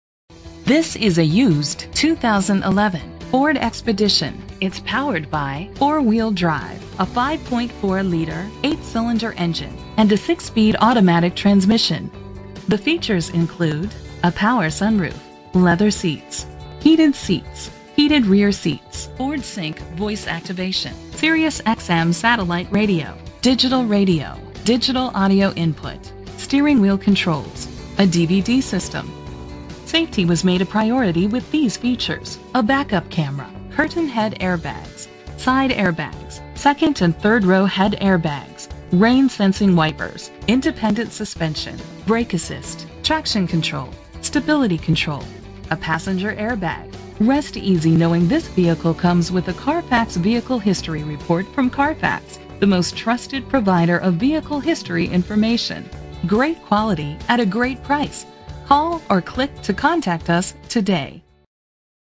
2011 Ford Expedition - Lafayette IN
Year: 2011
Make: Ford
Model: Expedition
Trim: Limited
Engine: 5.4 liter 8 cylinder 24 valve MPFI SOHC Flexible Fuel
Transmission: 6-Speed Automatic
Color: White
Mileage: 86247
Address:
2051 Sagamore Pkwy. S.